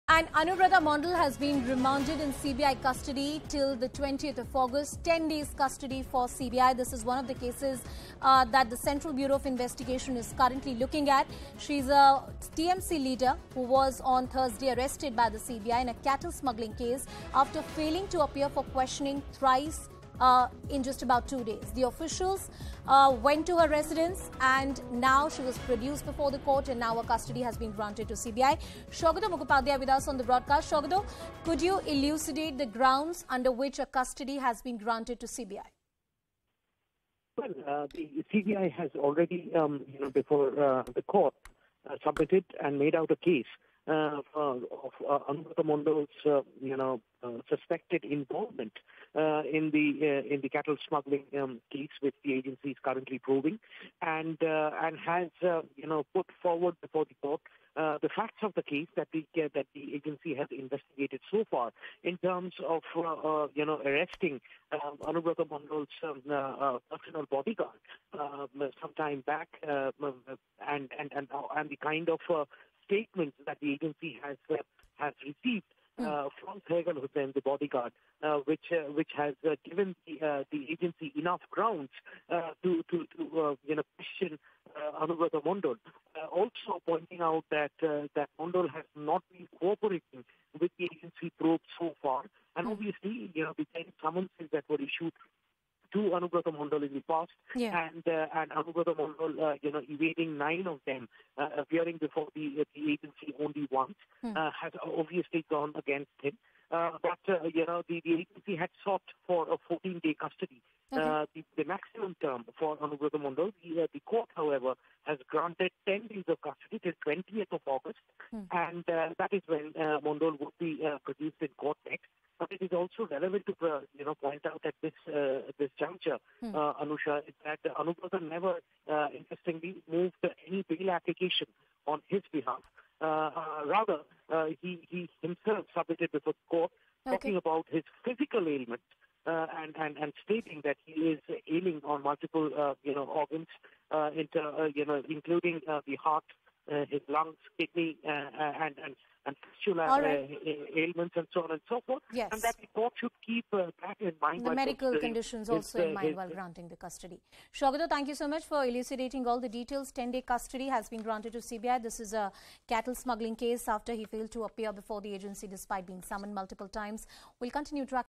Cattle Smuggling Case | CBI gets 10-day custody | Anubrata Mondal Arrested | CBI Probe | CNN News18
Trinamool Congress leader Anubrata Mondal was arrested by the Central Bureau of Investigation in a cattle smuggling case after failing to appear for questioning twice in three days. The team of CBI officials escorted by a huge contingent of central armed forces personnel on Thursday morning reached the Bolpur residence of the Trinamool Congress strongman

n18oc_breaking-news n18oc_world